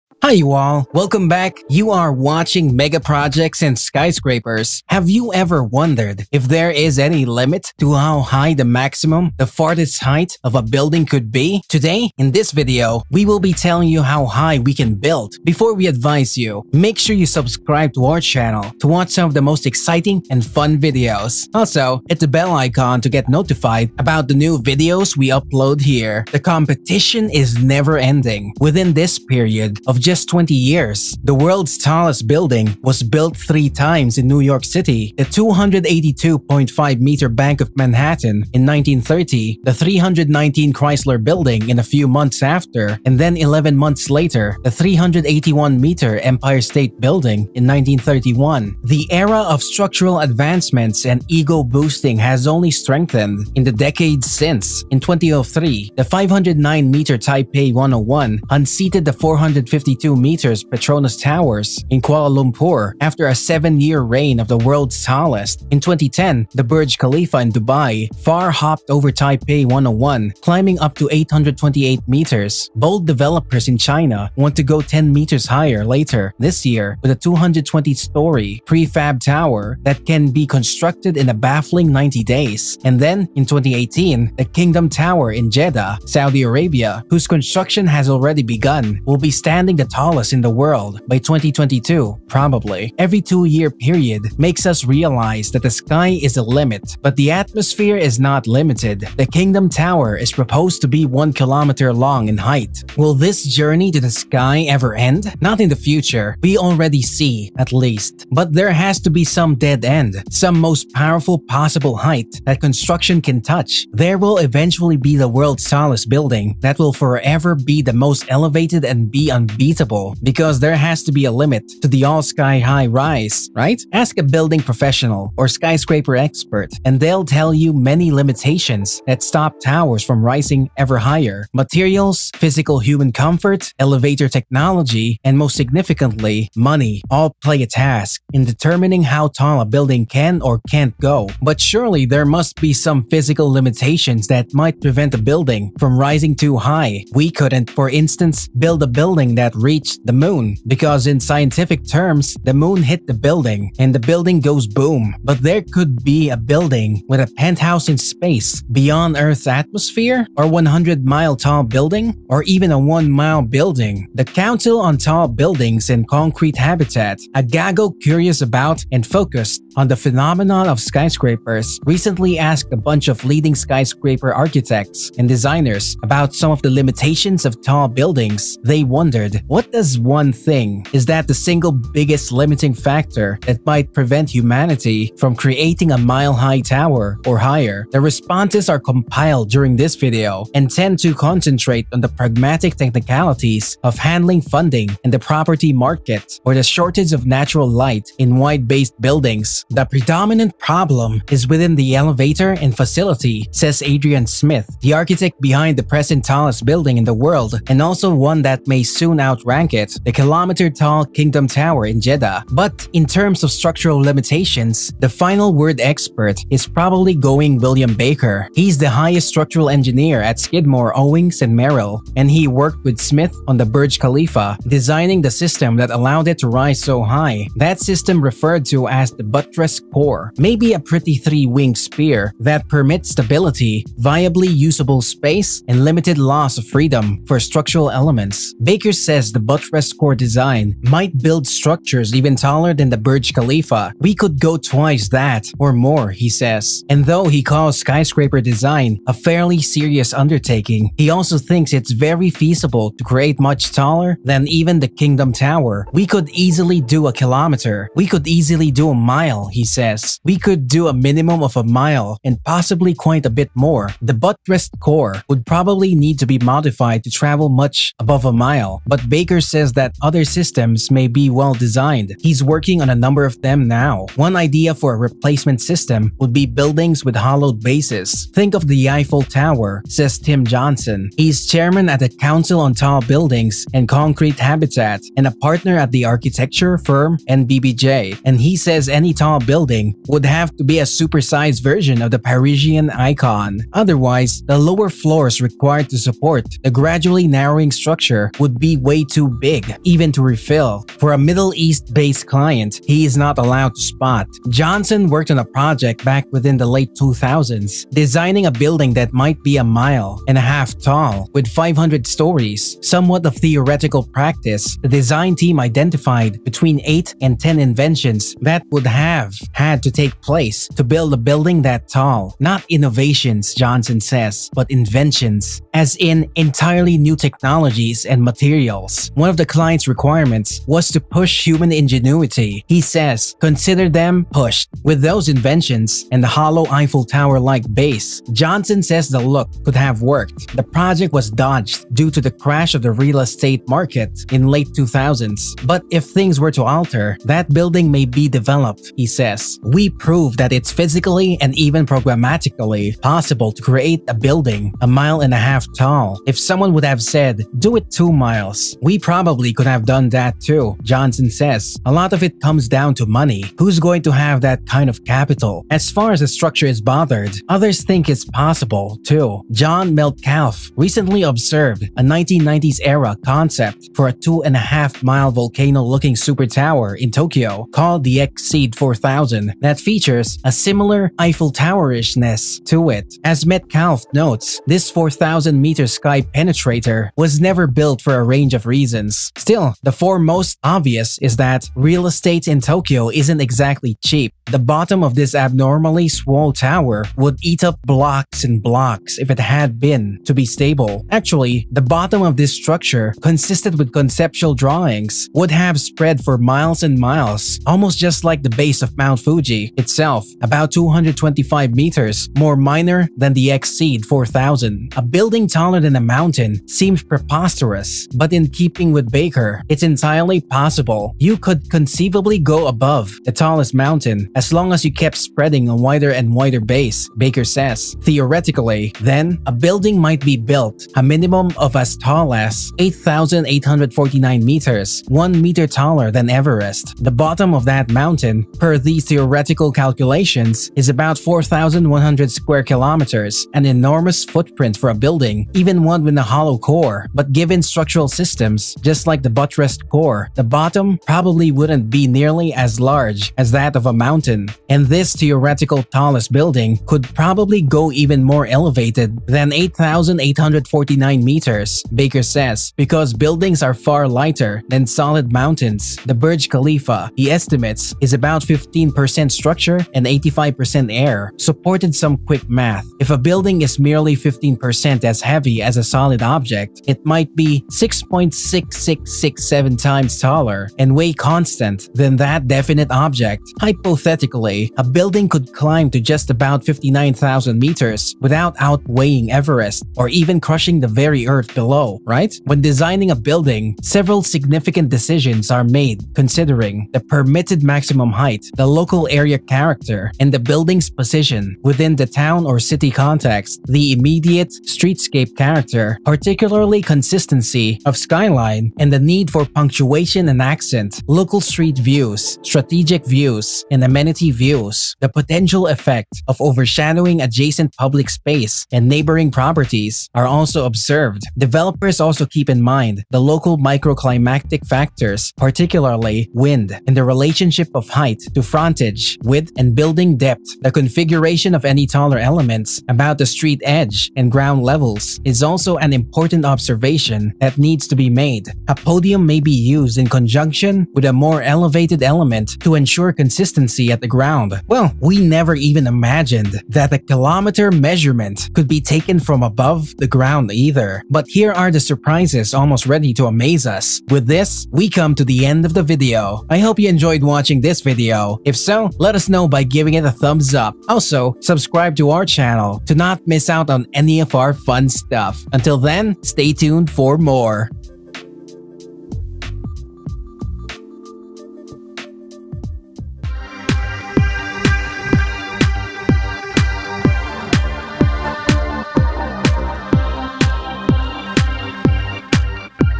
How High could we Build In The Future?
Have you ever wondered if there is any limit to how high the maximum, the farthest height of a building could be? Today in this video, we will be telling you how high we can build!

Why tall buildings are called skyscrapers?
Some buildings are called skyscrapers because they are of great height and have an iron or steel frame inside that supports its floors and walls. ... From that point on, tall buildings began to soar into the air, scraping the sky.